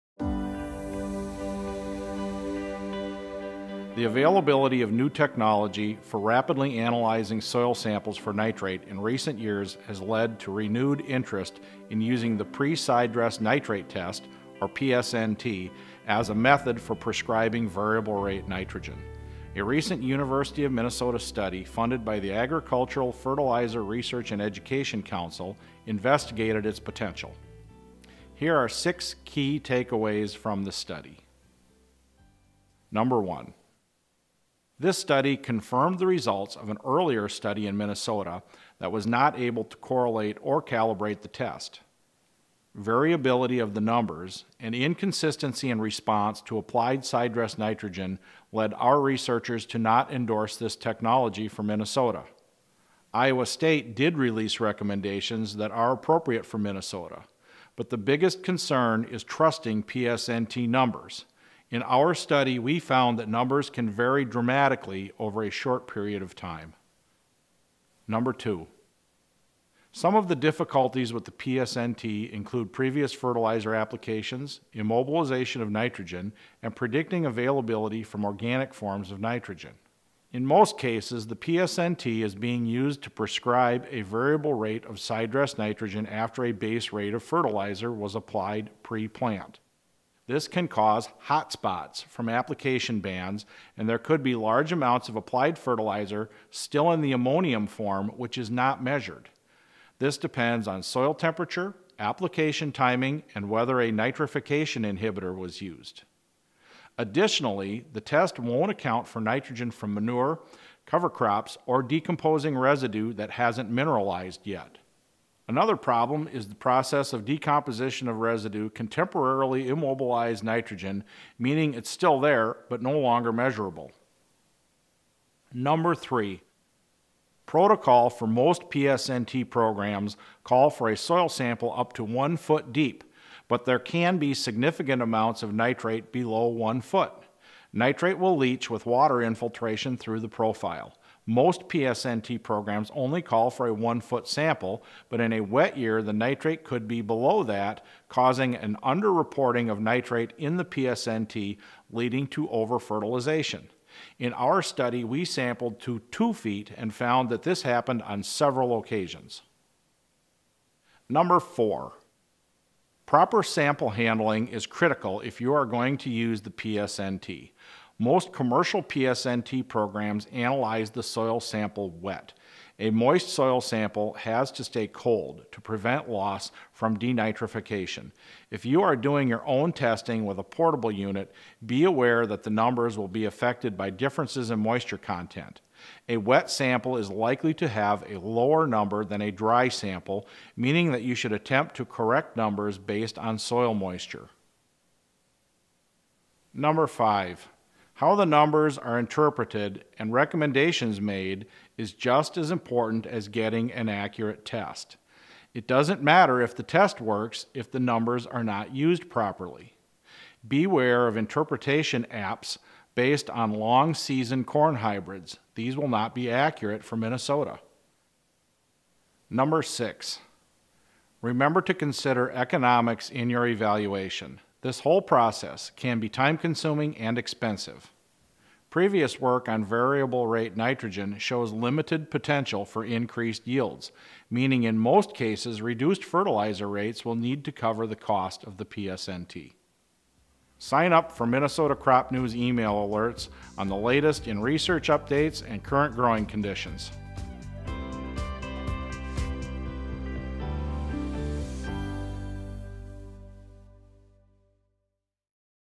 6 takeaways on the pre-sidedress nitrate test
Extension educator Brad Carlson discusses the results of a recent University of Minnesota study on the pre-sidedress nitrate test.